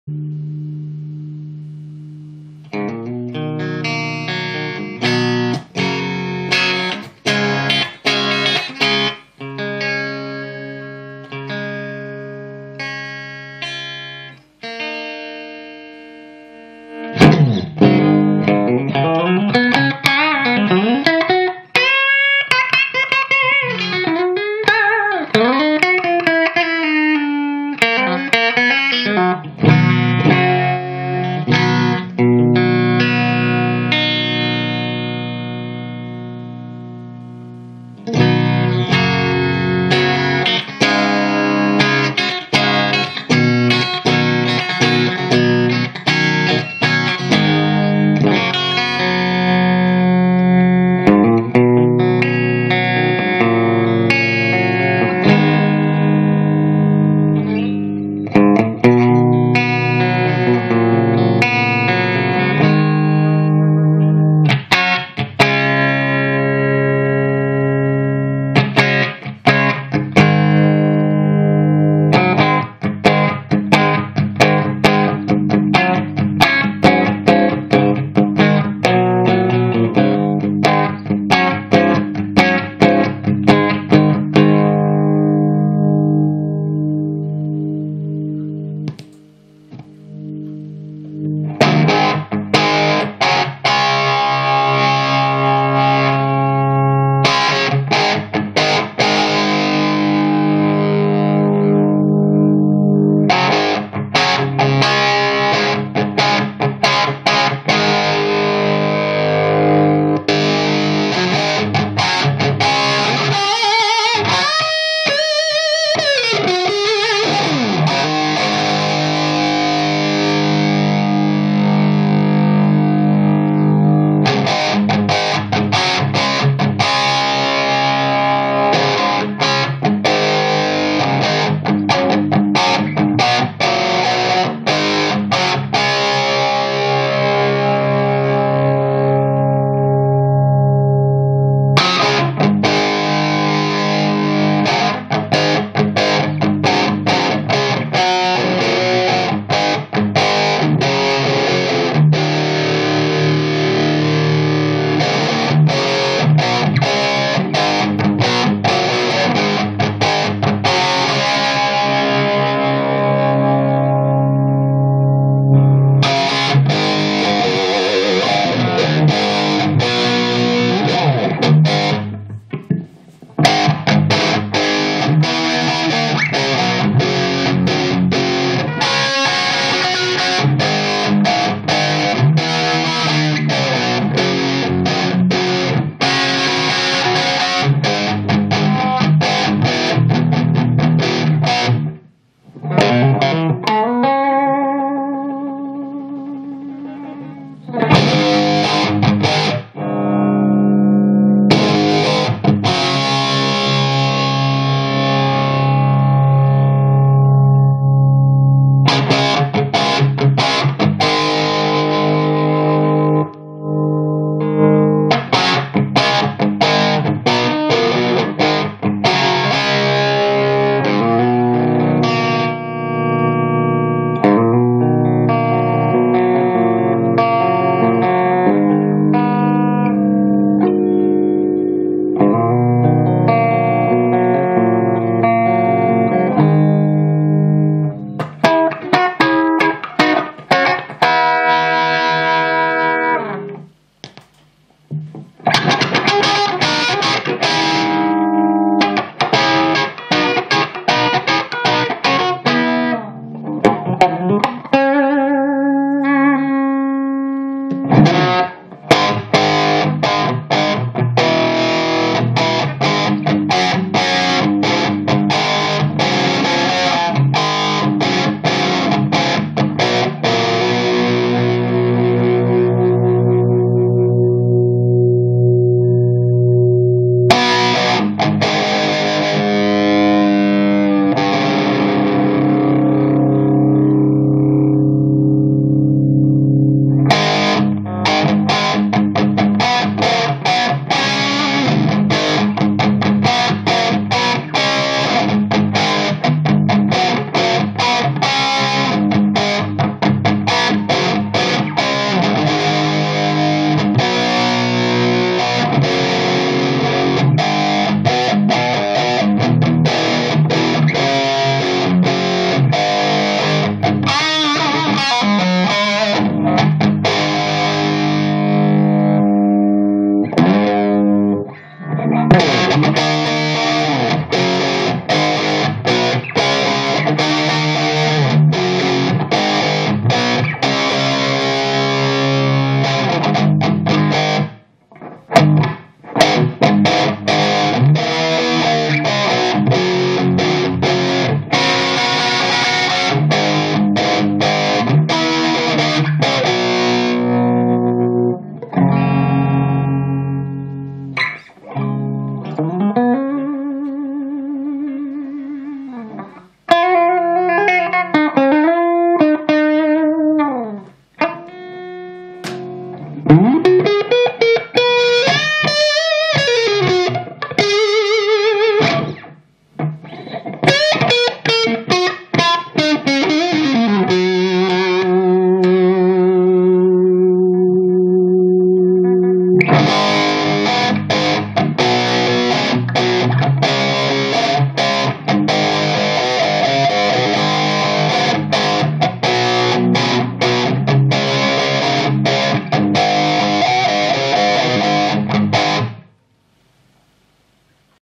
Marshall MA50H mod (2nd version / step)
This is another video taken with the videocam ... I think the amp is sounding a lil better now, but it is still very distorted at high frequencies. As the power amp section is very much "Marshall standard" I think it is the very little OT that causes it.

Anyway - the clean channel sounds pretty good to me, even with humbuckers.